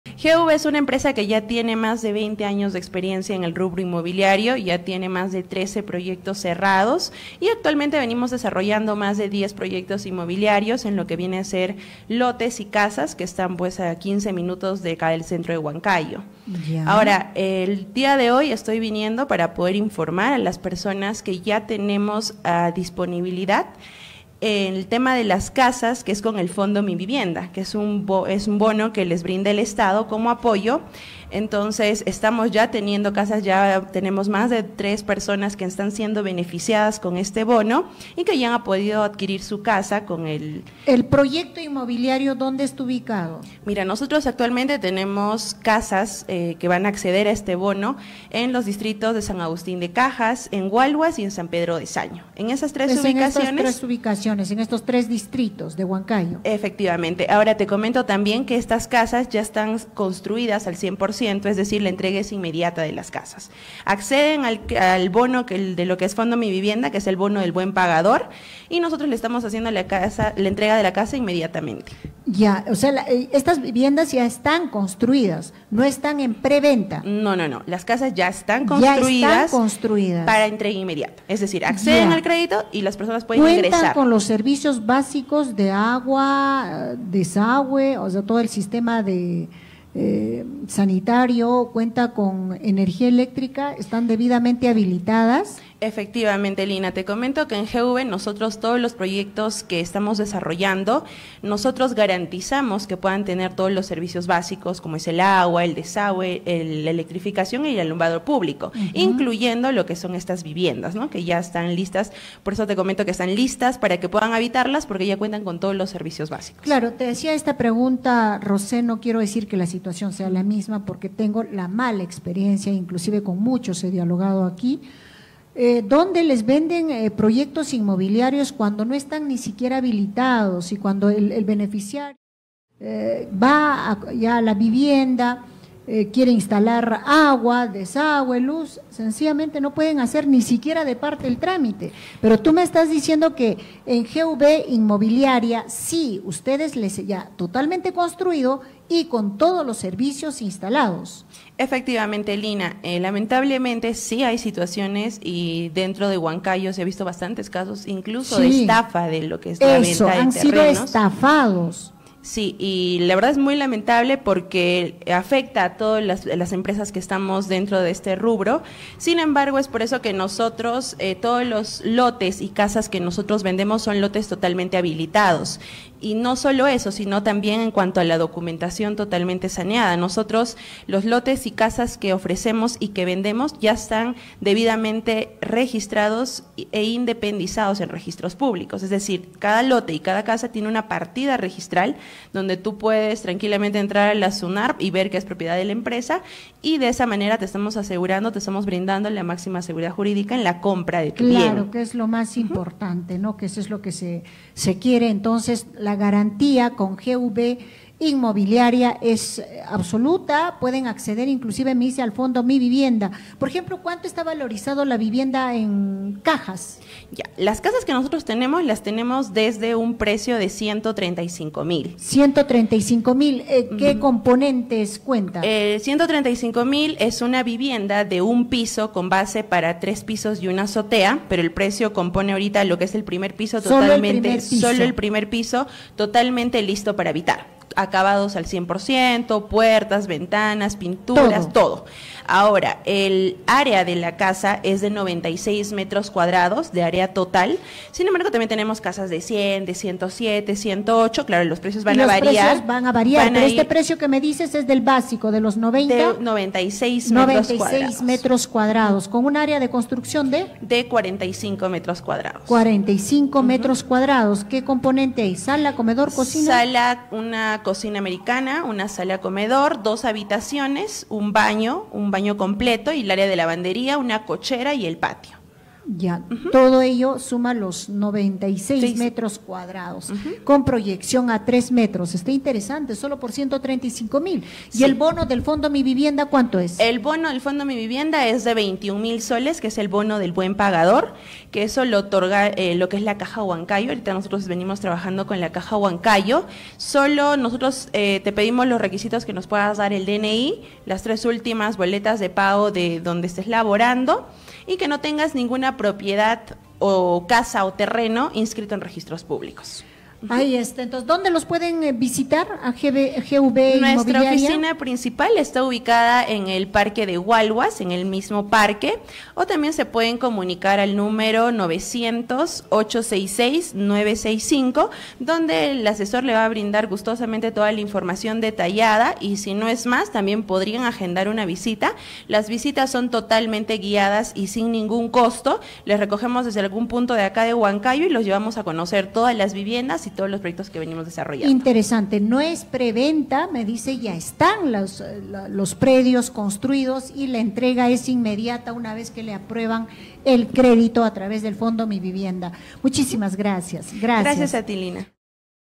AGRADECEMOS A RADIO EXITOSA POR LA ENTREVISTA
Agradecemos a Radio Exitosa Noticias🎤 por la entrevista que hoy nos realizaron. Compartimos detalles sobre la venta de 𝐂𝐀𝐒𝐀𝐒 🏠 con el Fondo Mi Vivienda, para que más adelante puedas tener un hogar propio.